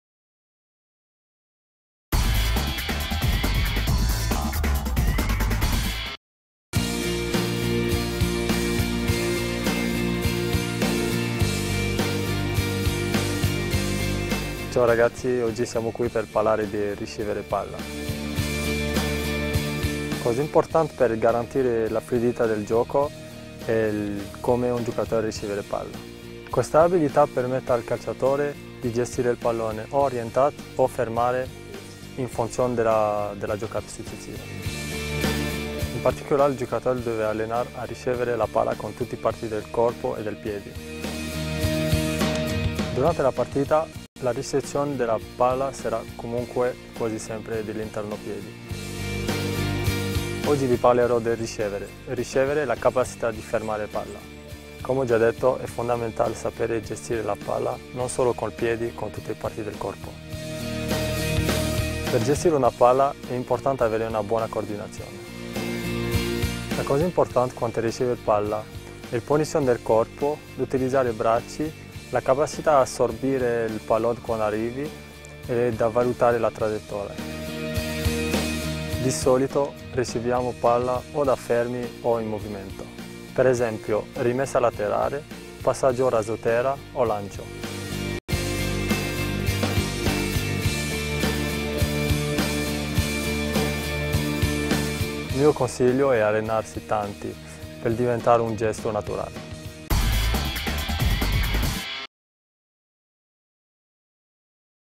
I segreti del calcio: Cyril Thèrèau e la ricezione della palla
I segreti del calcio : la ricezione della palla in un'azione di gioco. Cyril Thèrèau, top player del ChievoVerona, insegna a tutti i giovani gialloblu le tecniche migliori di ricezione della palla.